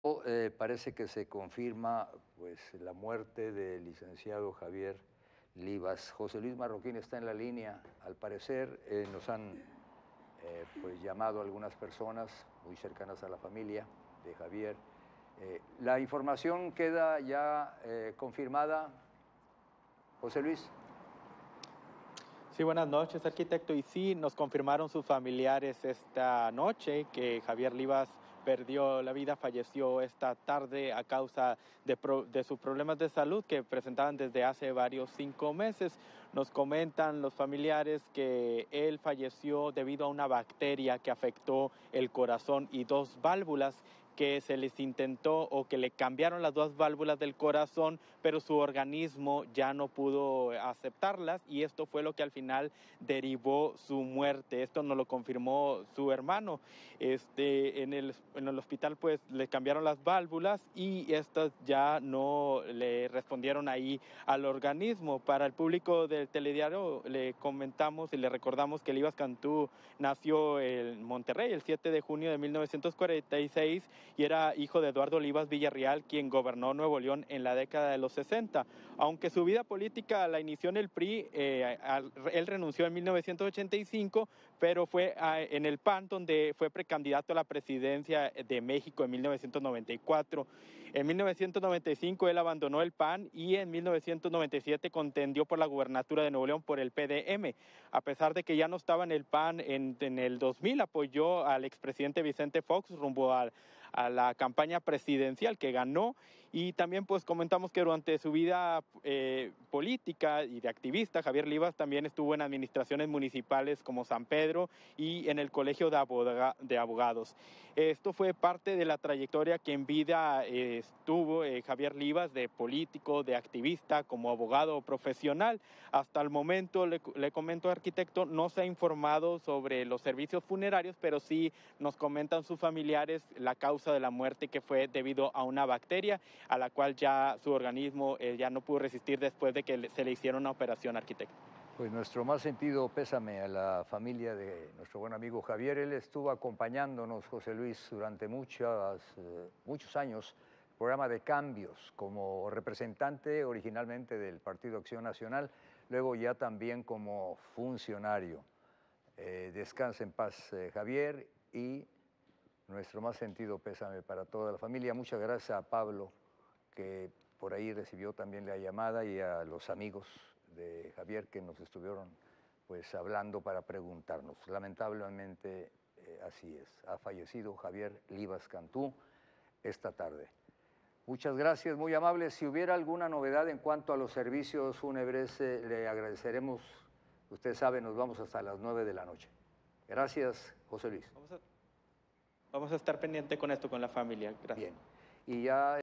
Muere el político regiomontano Javier Livas Cantú a los 76 años
Livas Cantú nació en Monterrey el 7 de junio de 1946 y era hijo de Eduardo Livas Villarreal, quien gobernó Nuevo León en la década de los sesenta.

Programación
Lunes a viernes:
Telediario Matutino de 05:00- 10:00
Telediario Mediodía de 12:00-15:00
Telediario Nocturno de 19:00- 21:00